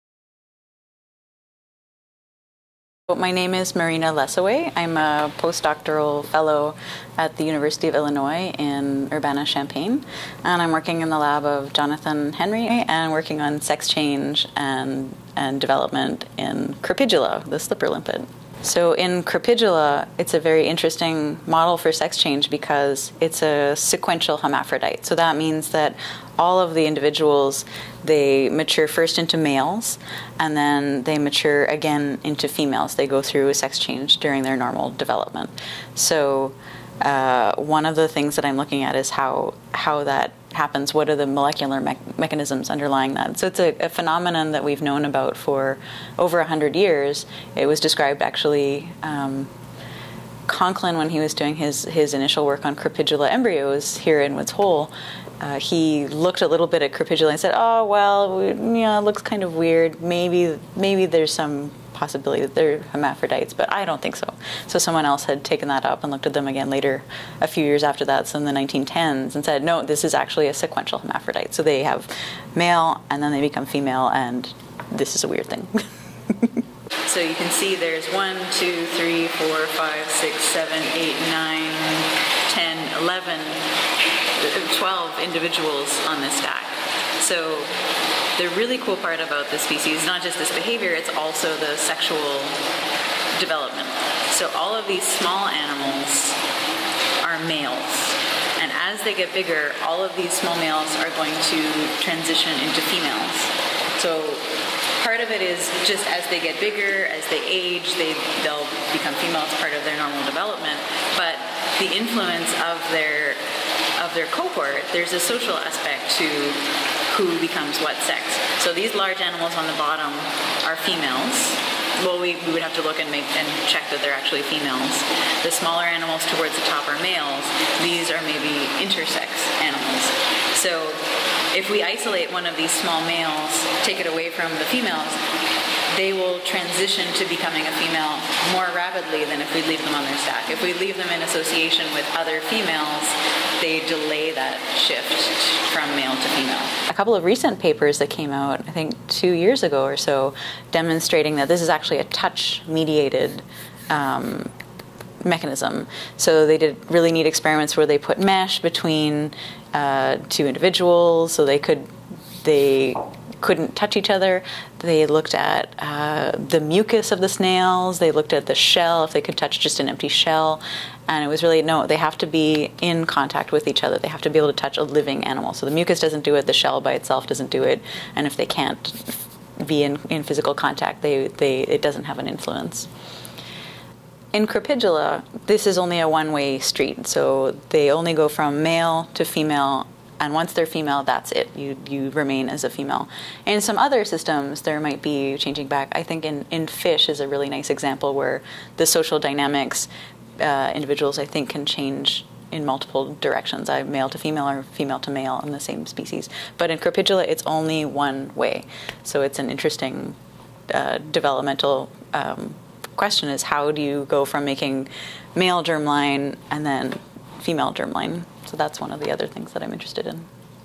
Meet the scientist working on the crazy sex life of slipper limpets
Maryna Lesoway from the University of Illinois at Urbana Champaign talks about the sex determination of the slipper limpet Crepidula fornicata